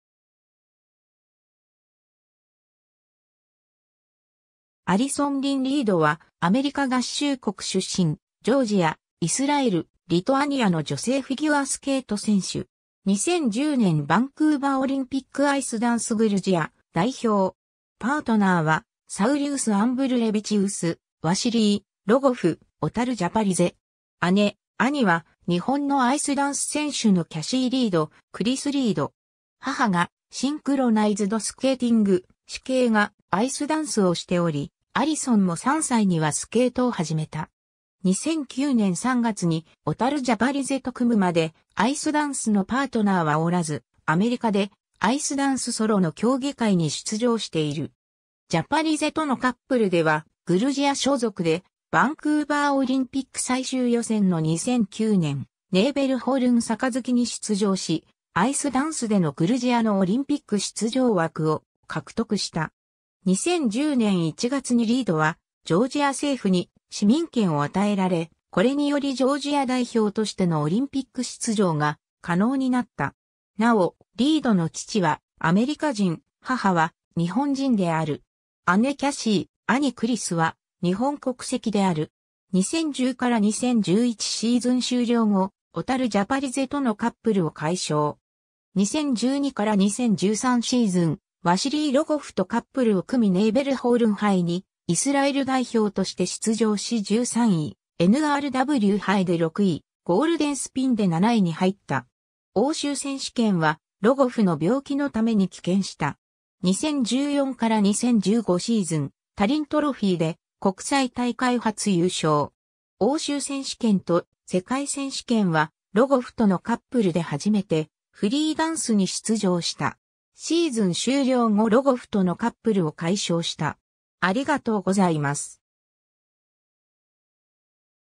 アリソン・リード (フィギュアスケート選手)
アリソン・リン・リード（英語: Allison Lynn Reed、グルジア語: ელისონ რიდი、1994年6月8日 - ）は、アメリカ合衆国出身、ジョージア（旧称グルジア）、イスラエル、リトアニアの女性フィギュアスケート(アイスダンス)選手。
2010年バンクーバーオリンピックアイスダンスグルジア代表。
パートナーはサウリウス・アンブルレヴィチウス、ワシリー・ロゴフ、オタル・ジャパリゼ。
姉、兄は日本のアイスダンス選手のキャシー・リード、クリス・リード。
 母がシンクロナイズドスケーティング、姉兄がアイスダンスをしており、アリソンも3歳にはスケートを始めた。
2009年3月にオタル・ジャパリゼと組むまでアイスダンスのパートナーはおらず、アメリカでアイスダンスソロの競技会に出場している。
 ジャパリゼとのカップルではグルジア所属でバンクーバーオリンピック最終予選の2009年ネーベルホルン杯に出場し、アイスダンスでのグルジアのオリンピック出場枠を獲得した。
 2010年1月にリードはジョージア政府に市民権を与えられ 、これによりジョージア代表としてのオリンピック出場が可能になった。
なおリードの父はアメリカ人、母は日本人である。
姉キャシー、兄クリスは日本国籍である。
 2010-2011シーズン終了後、オタル・ジャパリゼとのカップルを解消。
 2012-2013シーズン、ワシリー・ロゴフとカップルを組みネーベルホルン杯にイスラエル代表として出場し13位、NRW杯で6位、ゴールデンスピンで7位に入った。
欧州選手権はロゴフの病気のために棄権した。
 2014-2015シーズン、タリントロフィーで国際大会初優勝。
欧州選手権と世界選手権はロゴフとのカップルで初めてフリーダンスに出場した。
シーズン終了後ロゴフとのカップルを解消した。